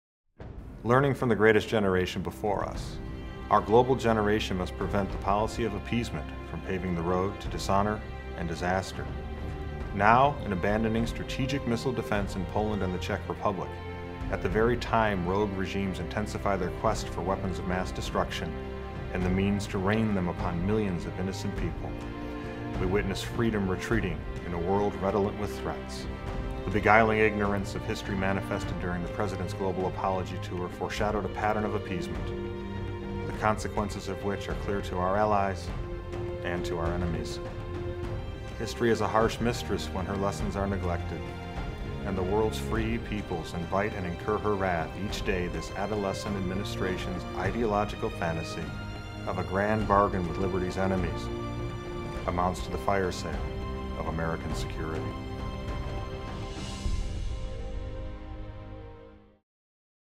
The Wakening Tapes: The Firesale of American Security
Citizens United is proud to launch a new web video featuring Representative Thaddeus McCotter on President Obama's Appeasement.